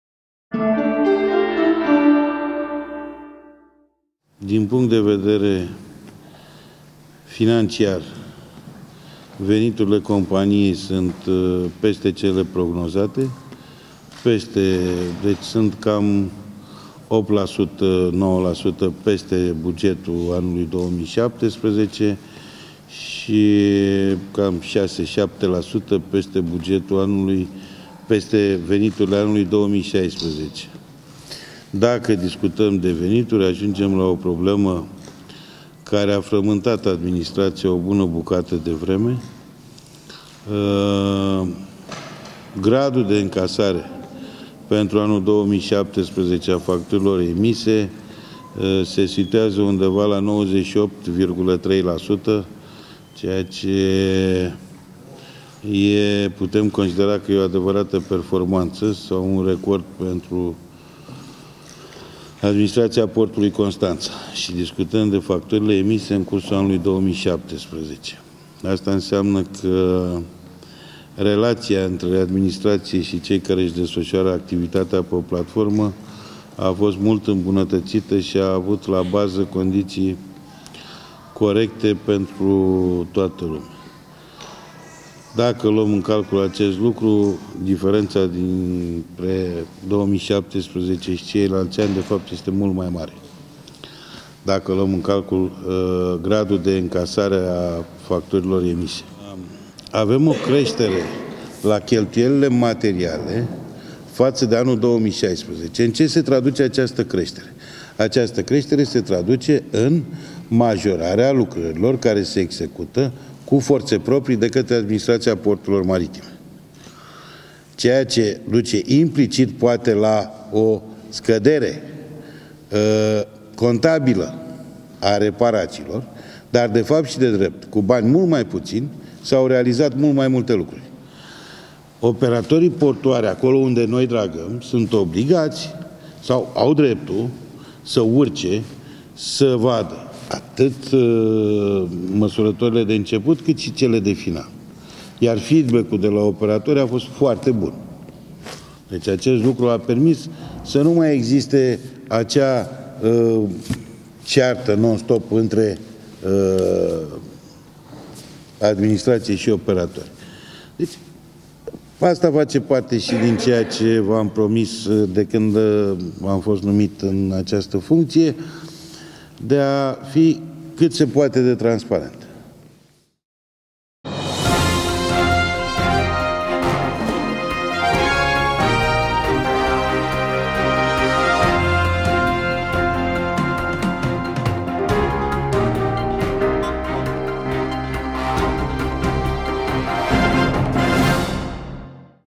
ctnews.ro | Nicolae Dan Tivilichi despre bilanțul CNAPMC în 2017
Nicolae Dan Tivilichi despre bilanțul CNAPMC în 2017